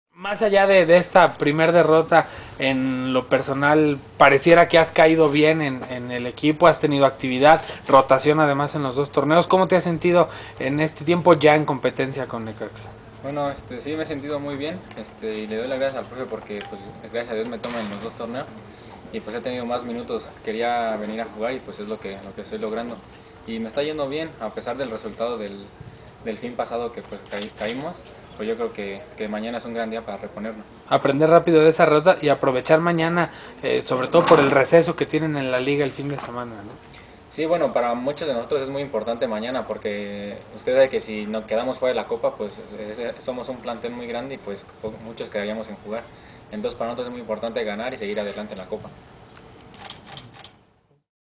Declaraciones de Lugiani Gallardo 30 de julio de 2012
El joven delantero del cuadro hidrocálido ve una gran oportunidad el día de mañana en el Torneo de Copa MX cuando los Rayos enfrenten en casa a Jaguares de Chiapas.